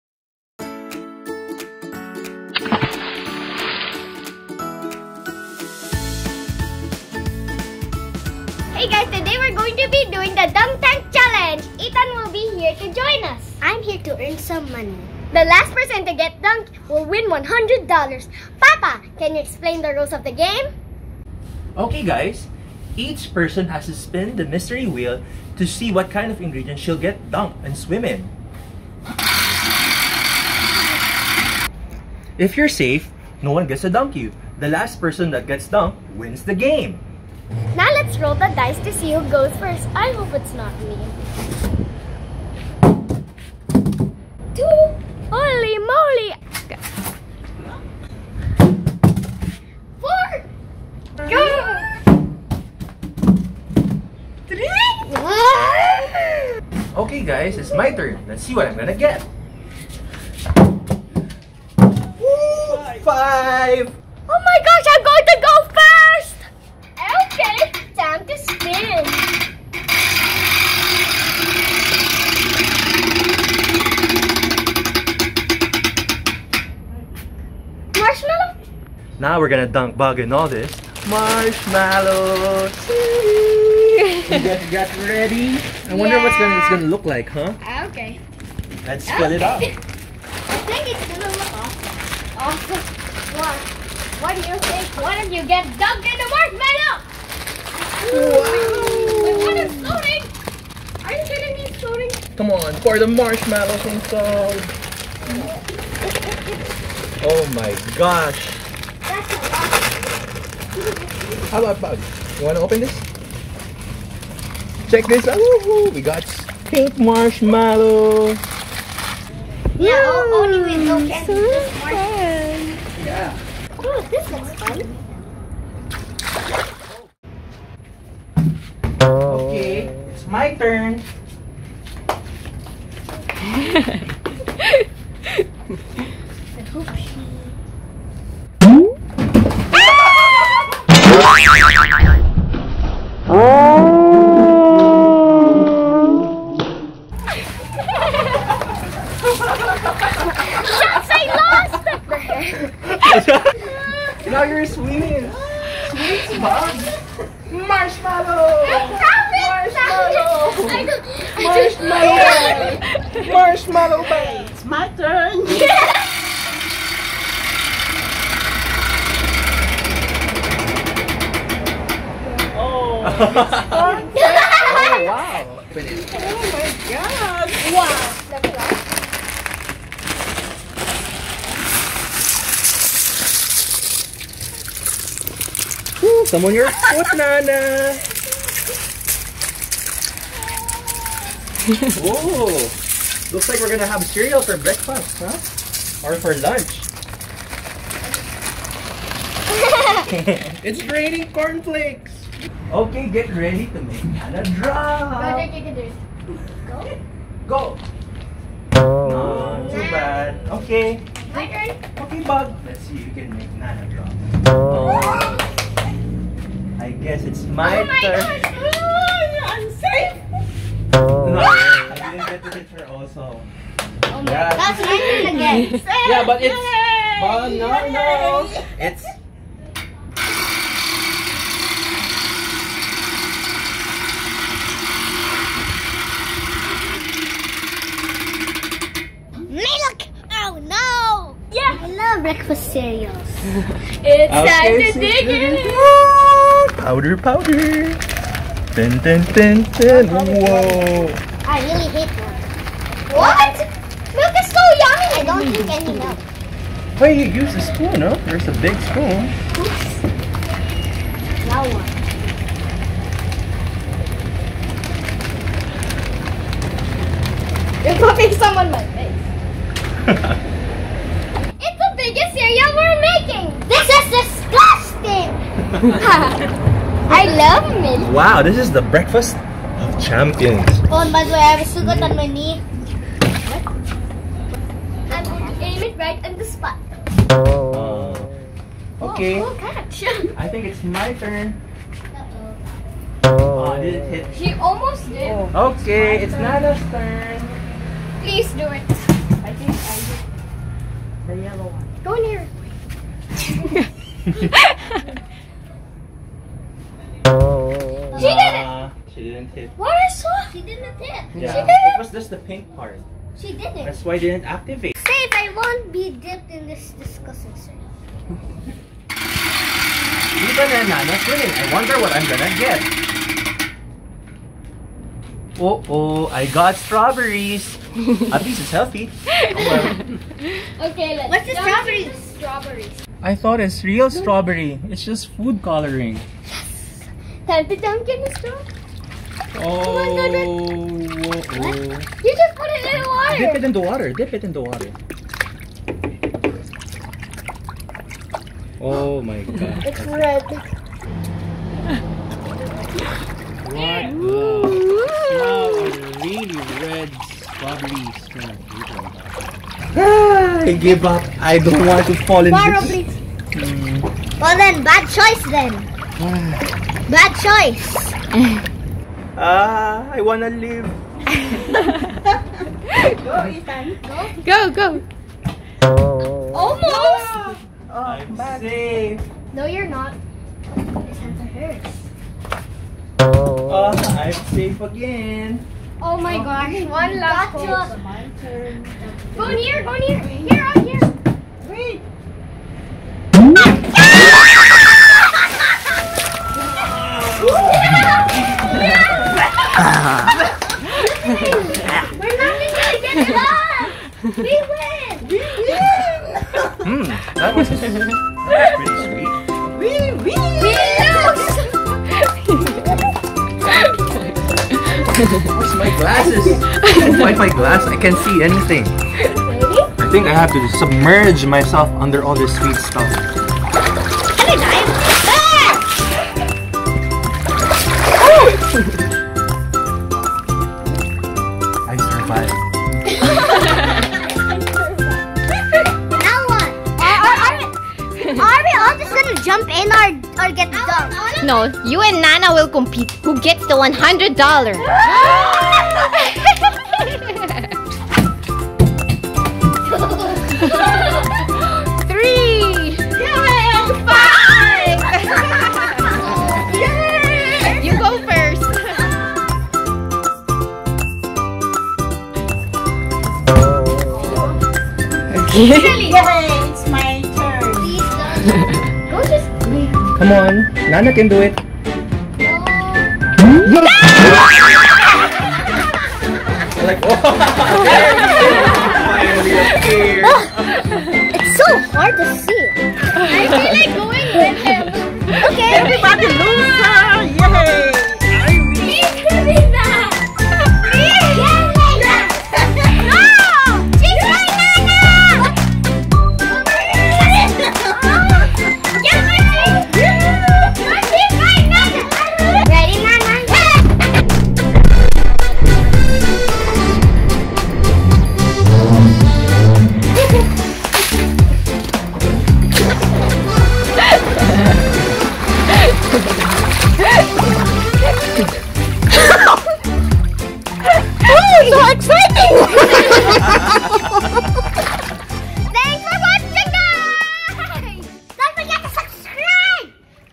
Dunk Tank Game with Bug's Family
Family fun game where you spin the wheel to see what you will get dunked in.